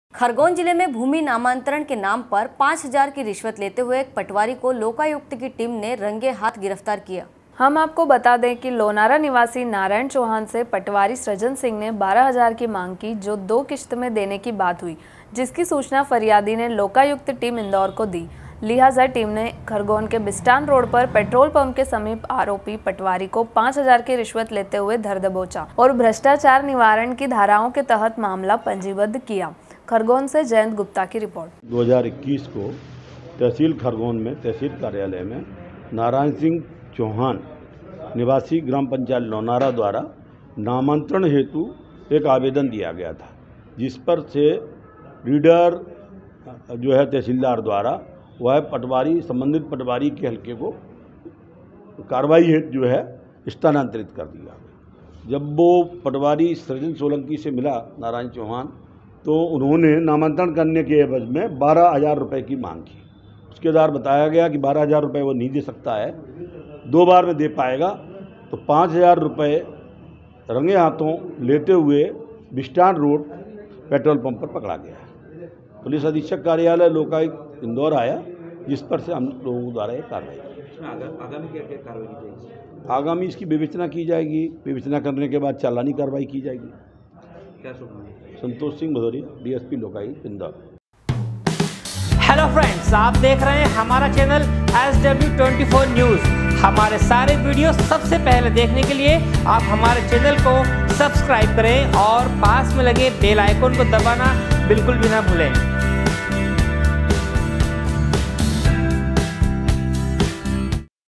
khargone- पाँच हजार की रिश्वत लेते एक पटवारी को लोकायुक्त की टीम ने रंगेहाथ गिरफ्तार किया
खरगोन जिले में भूमि नामांतरण के नाम पर पाँच हजार की रिश्वत लेते एक पटवारी को लोकायुक्त की टीम ने रंगेहाथ गिरफ्तार किया। खरगोन से जयंत गुप्ता की रिपोर्ट